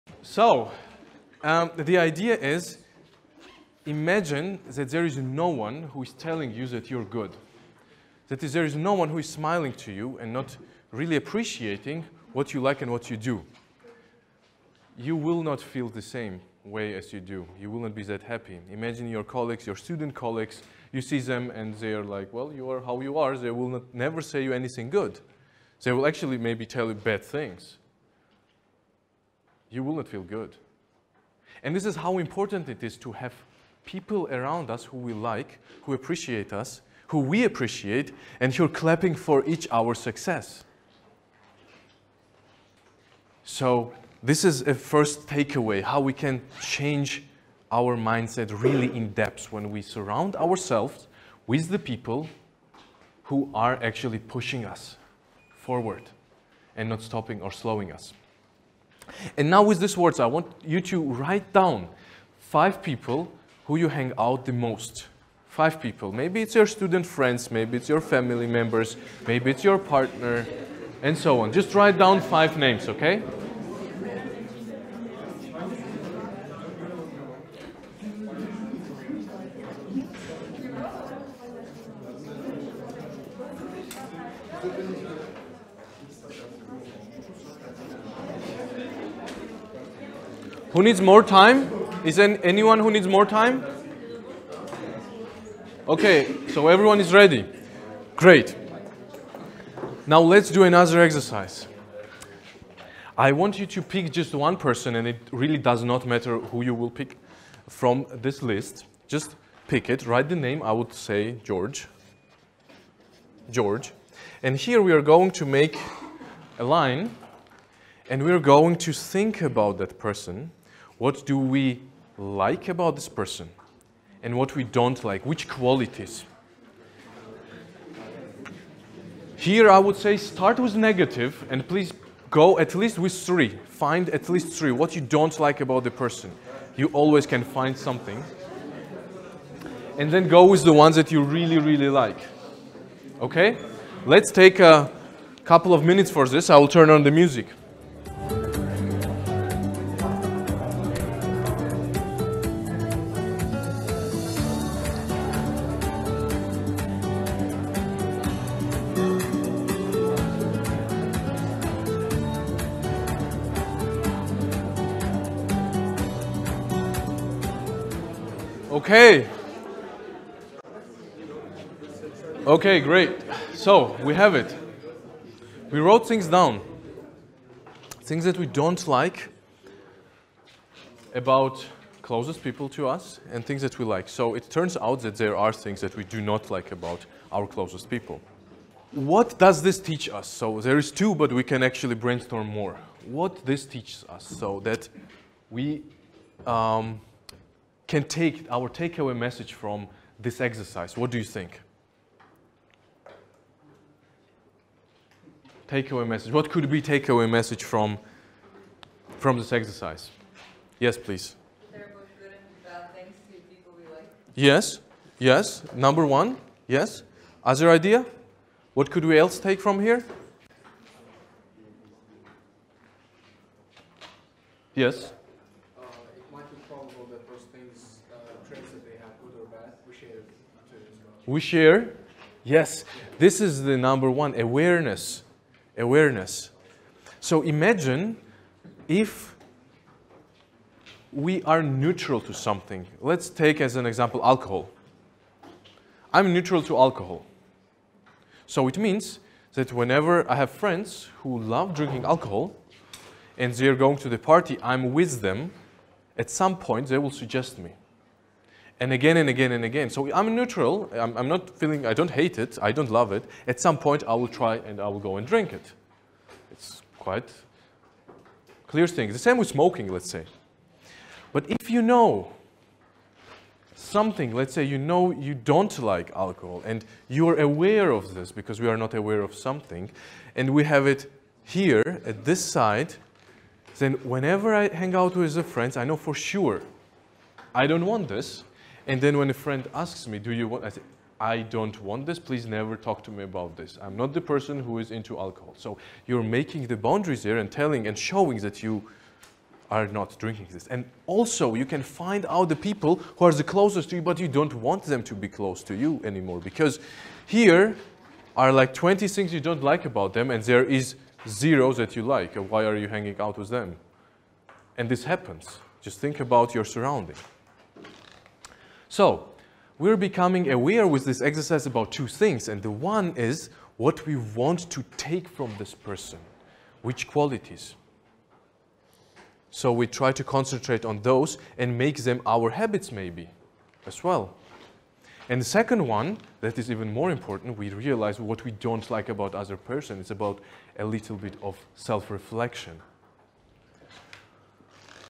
Mindset - How important are my friends? | Surrounding Pros and Cons Exercise
Have you ever thought how many people are surrounding you?
How many people around you do you actually like?
What do you like about them and what you hate about them?

It turns out that our surrounding influences our mindset, the way we think, the way we take actions and the way we make our decisions.

To be always the best version of yourself, take 10 minutes to do the exercise presented in this video and reflect on your  friends, family members, colleagues.